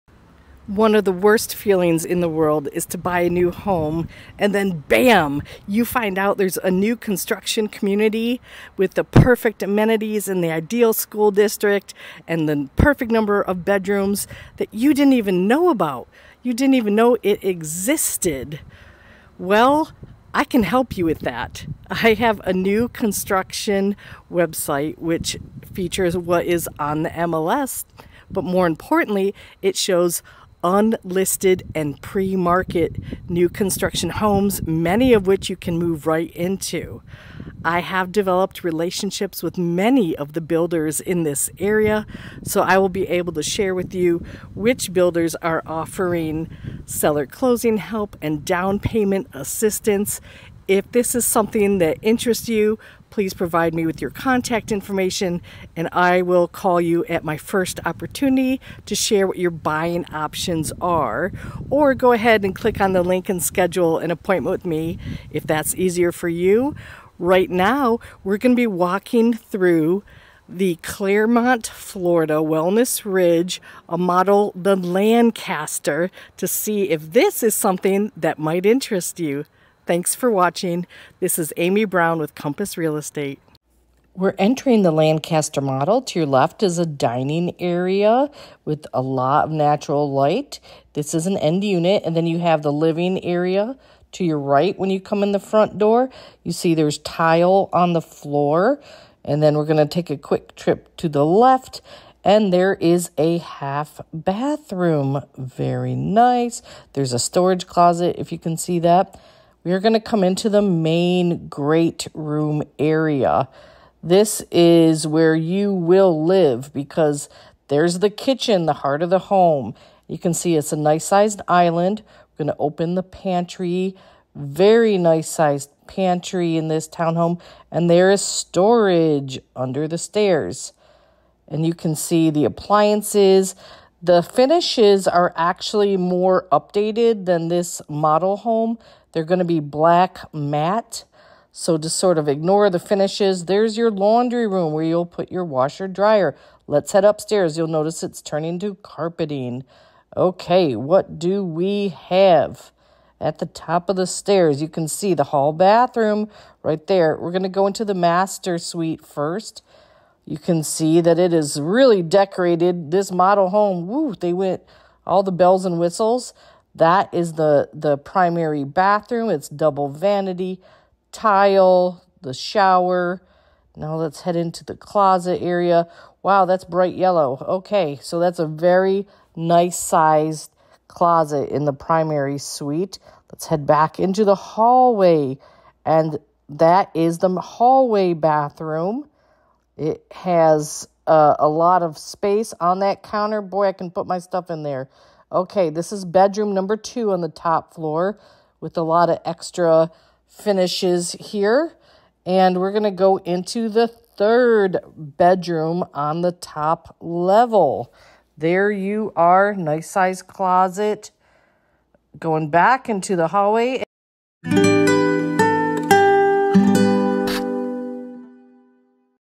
New Construction Home Tour 30 Minutes From Disney | Lennar Homes | Wellness Ridge | Clermont FL
New Construction Home Tour 30 Minutes From Disney | Lennar Homes | Wellness Ridge | Clermont, Florida |

Welcome to Wellness Ridge in Clermont, Florida built by Lennar Homes. Don’t miss the opportunity to get into a brand new construction home starting at approx $420,000 with Closing Help if you decide to work with their preferred lender, Lennar Mortgage. When a new phase of the community opens, prices will likely increase.

🤔Thinking of moving to Winter Garden, Florida? Let me help!

Amy Brown | JD MBA | CRS, SRES
Realtor @ COMPASS